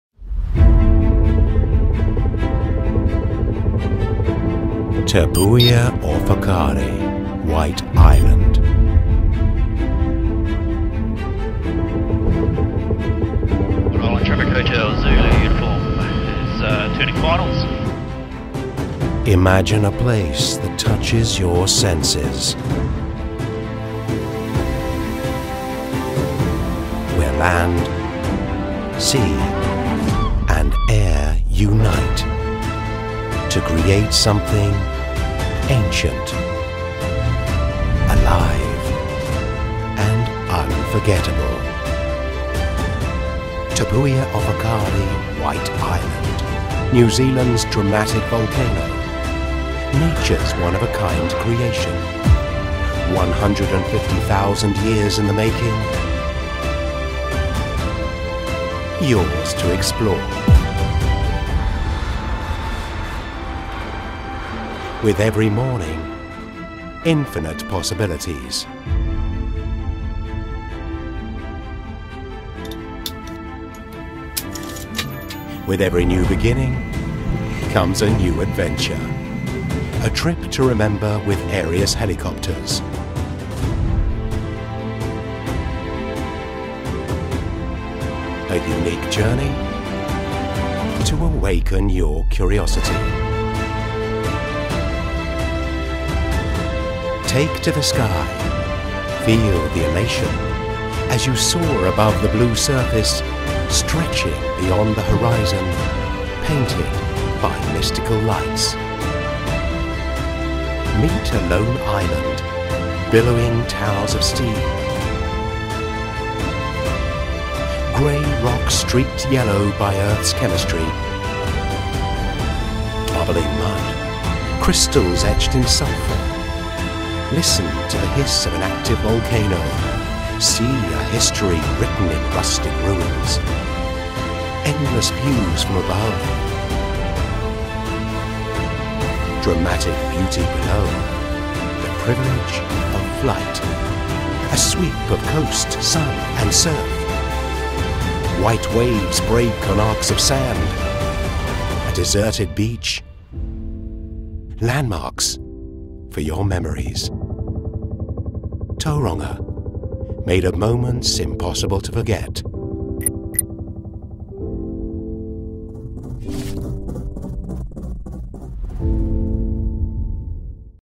Full Promo Video Aerius Helicopters
We are proud to show off our promo video!! This shows the Bay of Plenty from Above featuring New Zealand's ONLY active volcano White Island right in our back yard of Tauranga, the privilege of flight is an experience not to be missed.

specializing in White Island Flights or .
for scenic flights around the Bay of Plenty region